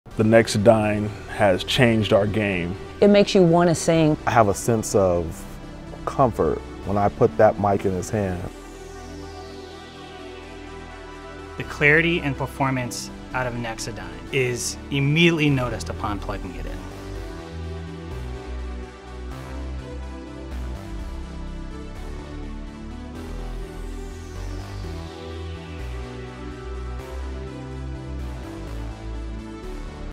Nexadyne™ Dynamic Vocal Microphone with Revonic Transducer Technology for Professional Performance
The ultimate dynamic vocal microphone is here! Combining state-of-the-art acoustic signal processing and exceptional polar pattern linearity, Nexadyne™ Dynamic Vocal Microphones confidently provide the exact vocal delivery required to every venue corner, every time.